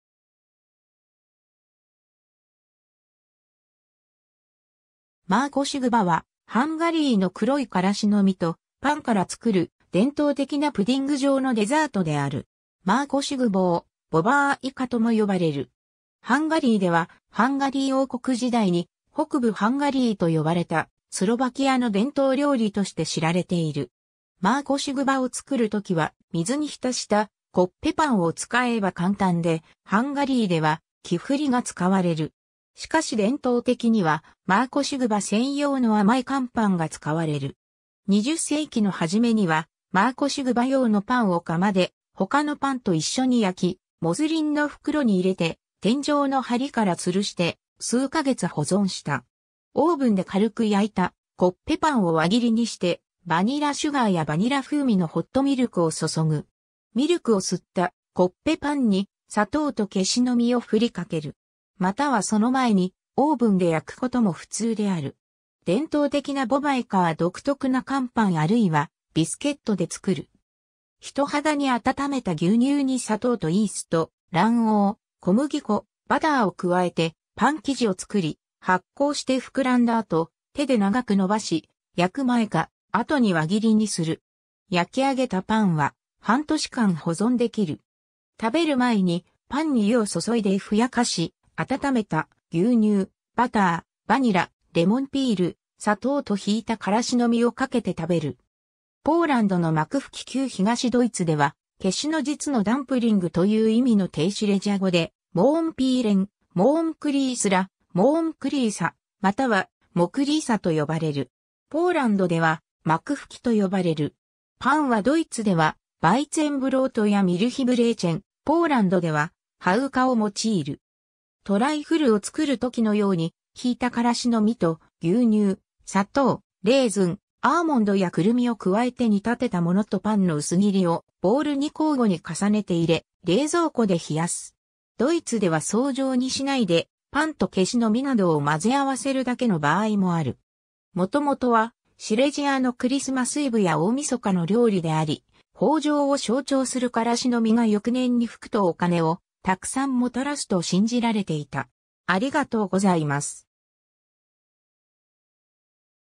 マーコシュ・グバ
マーコシュ・グバ（洪：Mákos Guba）はハンガリーの黒い芥子の実とパンから作る伝統的なプディング状のデザートである。
マーコシュ・グボー（mákos gubó）、ボバーイカ（bobájka）とも呼ばれる。
ハンガリーでは、ハンガリー王国時代に北部ハンガリーと呼ばれたスロバキアの伝統料理として知られている。
 マーコシュ・グバを作るときは、水に浸したコッペパンを使えば簡単で、ハンガリーではキフリが使われる。
しかし伝統的にはマーコシュ・グバ専用の甘い乾パンが使われる。
20世紀の始めにはマーコシュ・グバ用のパンを釜で他のパンといっしょに焼き、モスリンの袋に入れて天井の梁から吊るして数ヶ月保存した。
 オーブンで軽く焼いたコッペパンを輪切りにして、バニラシュガーやバニラ風味のホットミルクを注ぐ。
ミルクを吸ったコッペパンに、砂糖と芥子の実をふりかける。
またはそのまえにオーブンで焼くことも普通である。
 伝統的なボバイカは独特な乾パンあるいはビスケットで作る。
人肌に温めた牛乳に砂糖とイースト、卵黄、小麦粉、バターを加えてパン生地を作り、発酵して膨らんだ後、手で長くのばし、焼く前か後に輪切りにする。
焼き上げたパンは半年間保存できる。
食べる前にパンに湯を注いでふやかし、温めた牛乳、バター、バニラ、レモンピール、砂糖と挽いた芥子の実をかけて食べる。
 ポーランドのマクフキ 旧東ドイツでは「芥子の実のダンプリング」という意味の低シレジア語でモーンピーレン (Mohnpielen)、モーンクリースラ（Mohnkließla）、モーン＝クリーサ（Mohn-Kließa）またはモクリーサ（Mokließa）と呼ばれる。
ポーランドではマクフキ（Makówki）と呼ばれる。
 パンはドイツではヴァイツェンブロートやミルヒブレーチェン（牛乳でこねたロールパン）、ポーランドではハウカを用いる。
トライフルを作るときのように、挽いた芥子の実と牛乳、砂糖、レーズン、アーモンドやクルミを加えて煮立てたものとパンの薄切りをボウルに交互に重ねて入れ、冷蔵庫で冷やす。
ドイツでは層状にしないでパンと芥子の実などを混ぜ合わせるだけの場合もある。
 元々はシレジアのクリスマス・イヴや大晦日の料理であり、豊穣を象徴する芥子の実が翌年に福とお金をたくさんもたらすと信じられていた。